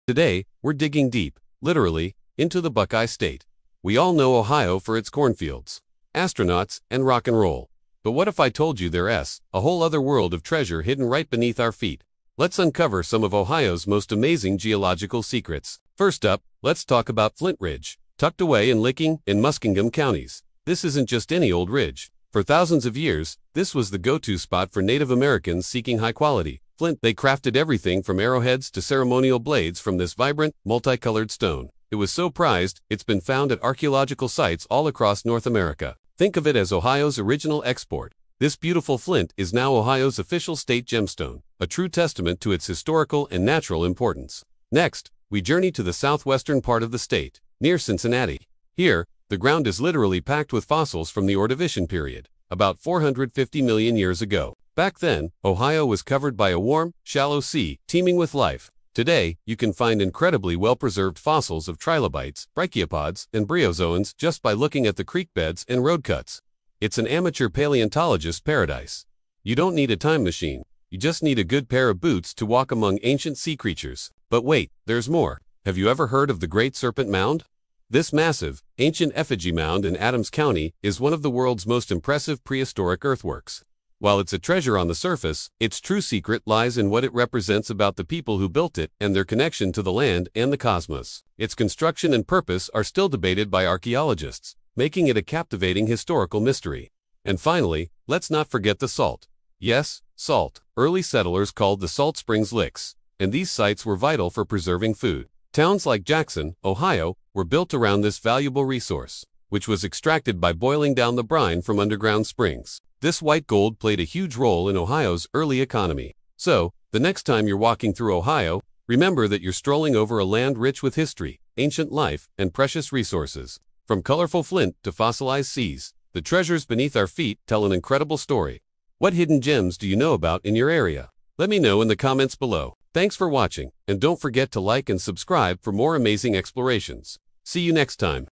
What is Buried Under Ohio? (It Was Once an Ancient Sea)
We know Ohio for cornfields and astronauts, but hidden beneath the soil lies a treasure trove of geological history.

Did you know Ohio used to be a tropical sea? Today, we dig deep into the Buckeye State’s secrets:

    Flint Ridge: The source of the colorful stone that Native Americans traded across the continent.

    The Fossil Coast: Why Cincinnati is a paradise for finding 450-million-year-old sea creatures.

    Serpent Mound: The mysterious prehistoric earthwork that aligns with the cosmos.

    White Gold: How ancient salt springs built Ohio's early economy.

Join us as we uncover the history written in the rocks right under our feet.